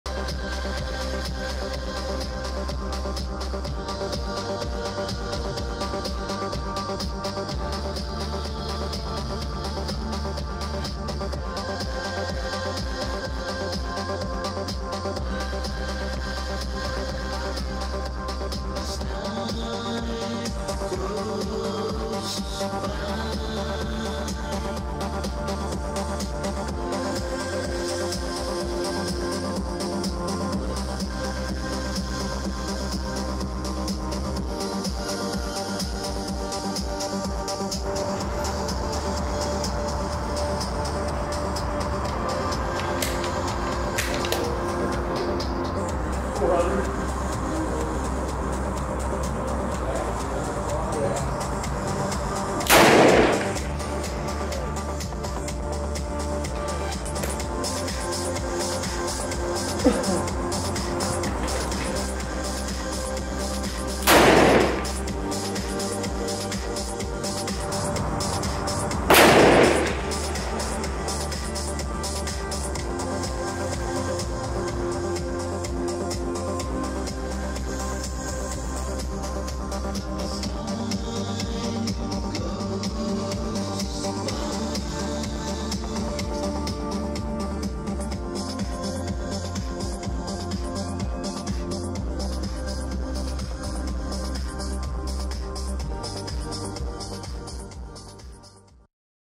300 Norma Mag 210 Berger Bullets
A custom 300 Norma Mag working on initial load data with a magnetospeed chronograph